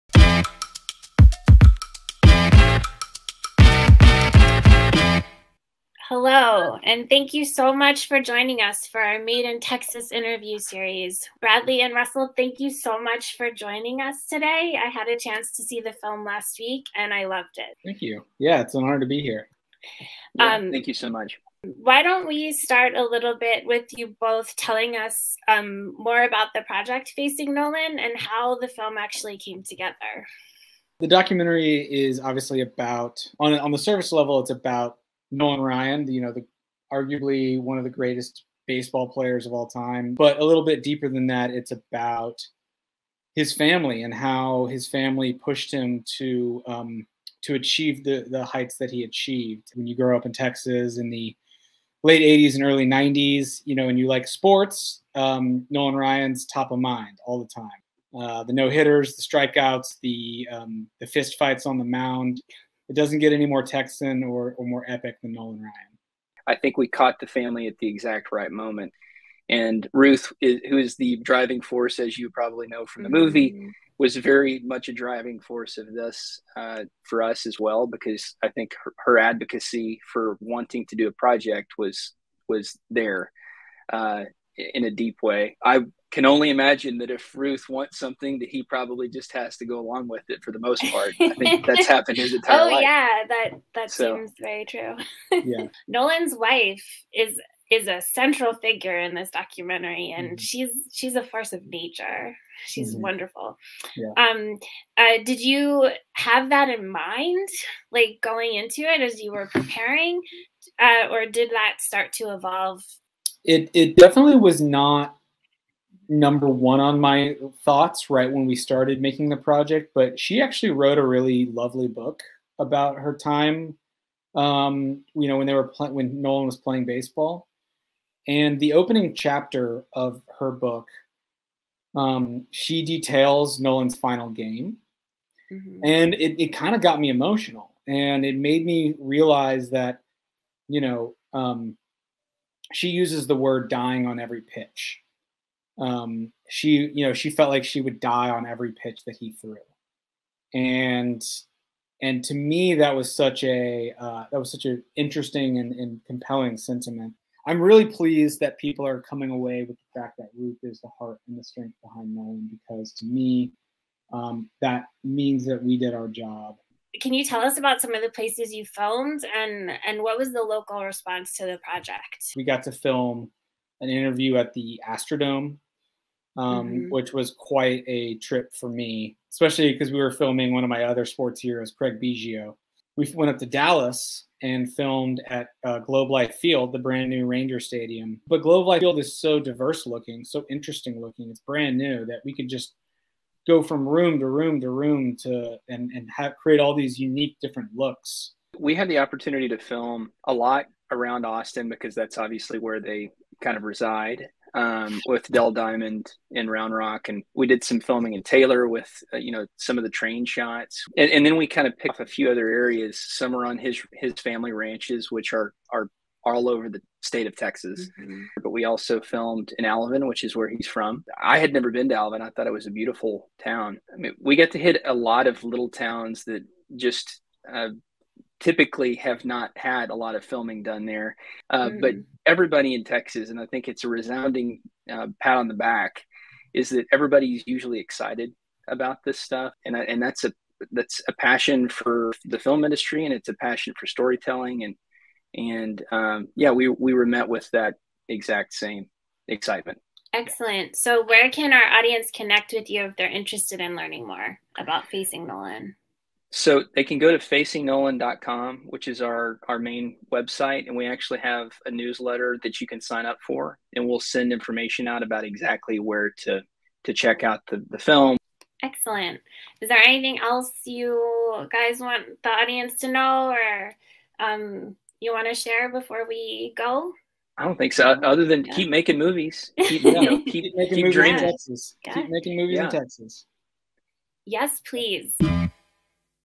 Facing Nolan filmmakers, Bradley Jackson & Russell Wayne Groves | Made in Texas
"It doesn't get any more Texan or more epic than Nolan Ryan," director Bradley Jackson told us while describing the process of documenting the life & career of legendary pitcher Nolan Ryan in SXSW-selection "Facing Nolan".

Hear from Bradley and producer Russell Wayne Groves as they discuss their filmmaking process, working with Nolan Ryan and more in our latest Made in Texas interview.

Want to learn more about the film and all the talented people involved in making it?